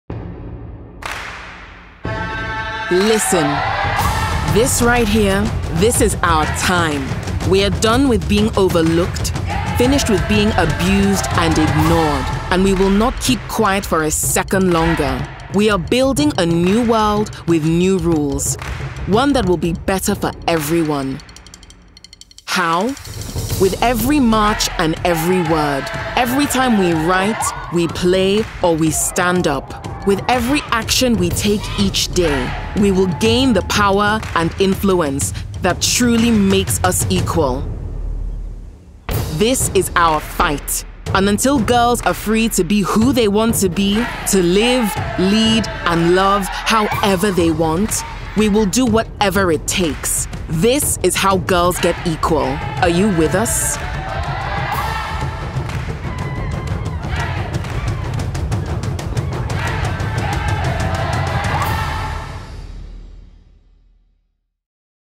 This is how Girls Get Equal
GirlsGetEqual is campaigning for a world where girls can live, love and lead without fear or discrimination.

Girls want a world where they can live freely without fear of attack. Where they can see themselves equally represented at the highest levels. Where they are treated with respect, not as sex objects. It’s what we all want.

Are you with us?